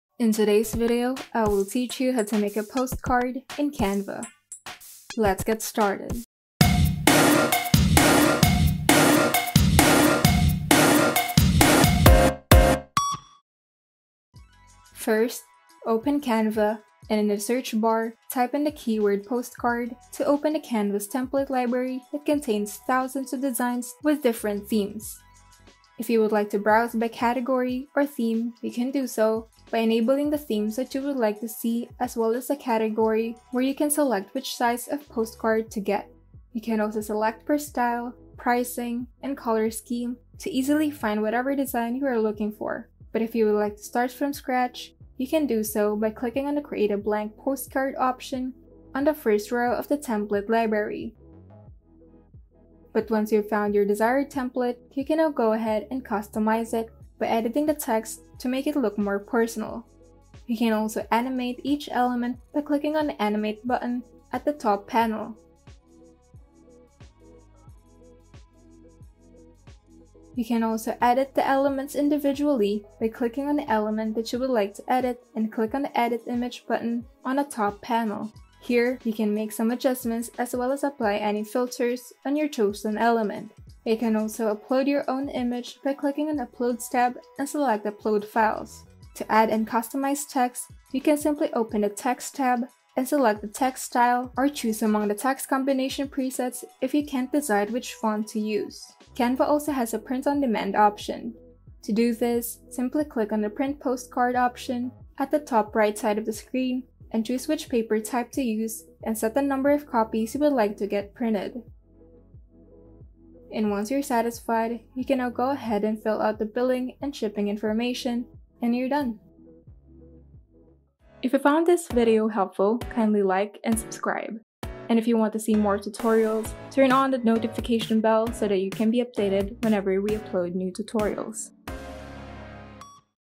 HOW-TO: Make Awesome 💌 POSTCARDS 💌 for Any Occasion in Canva! (4,000+ Templates)
Make a postcard at home for ANY Occasion!  Whether you want to make a travel poscard, business, real estate, church, or whatever the reason - Canva makes it simple with over 4,000 templates to choose from.  Watch this tutorial to see how you can get started!

00:00 (Intro) Making a Postcard in Canva
00:15 Choosing a Template
00:52 Customizing Your Postcard
01:09 Editing the Elements & Text
01:38 Print on Demand Option